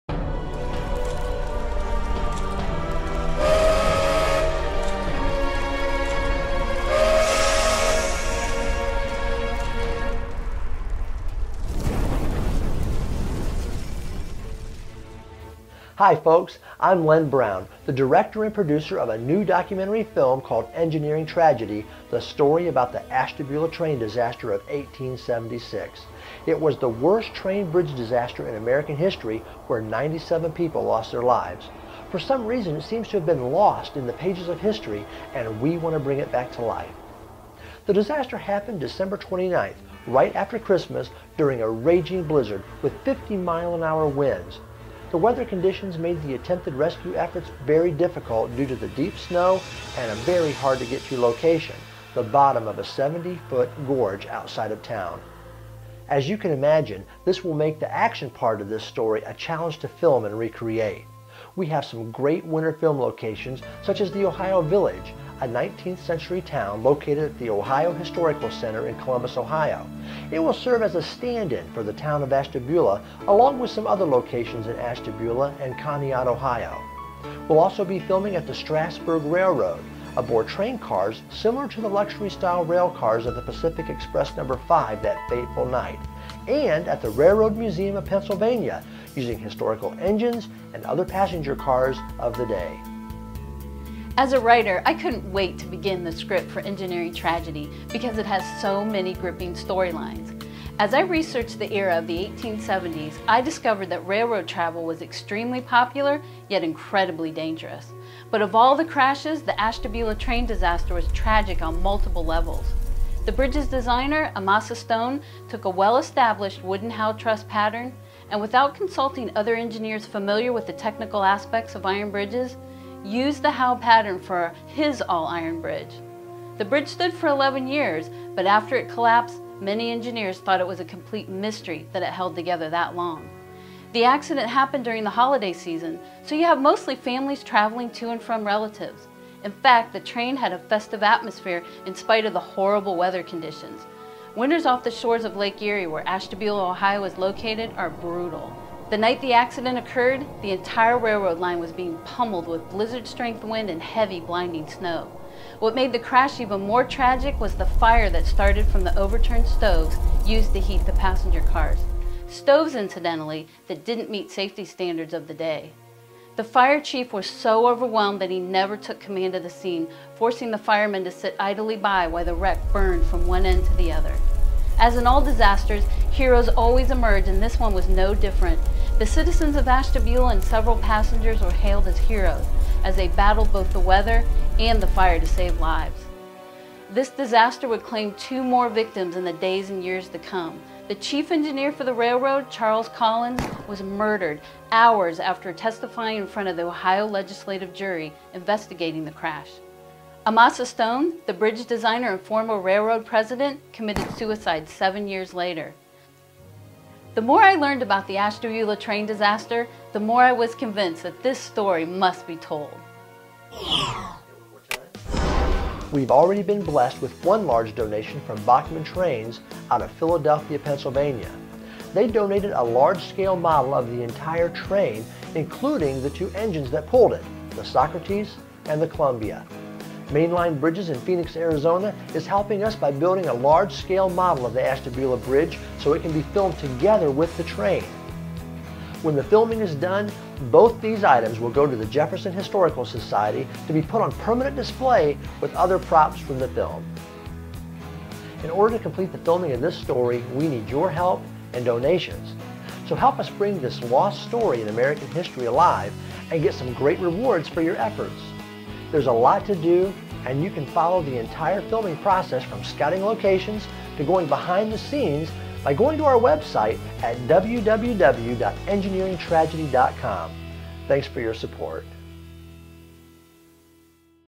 Ashtbula Train Disaster Documentary Fundraiser
This is the story of the Ashtabula Train Disaster. This is the fundraising video for a new documentary film entitled "Engineering Tragedy: The Ashtabula Train Disaster" being filmed in 2013. This film is for PBS and will be distributed to all PBS member stations through NETA and APT. Our presenting PBS station is WQLN in Erie, PA.